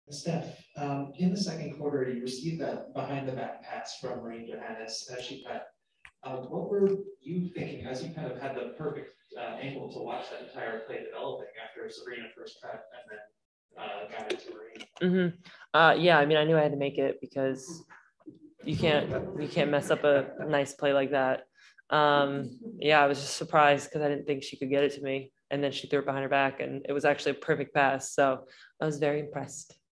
Stef Dolson on Marine Johannès Behind-the-Back Assist 6-12-22
In the second quarter of the New York Liberty’s 88-86 loss to the Chicago Sky, Marine Johannès threw a no-look behind-the-back pass to Stef Dolson, who hit the open three. I asked Stef about that pass after the game, for Winsidr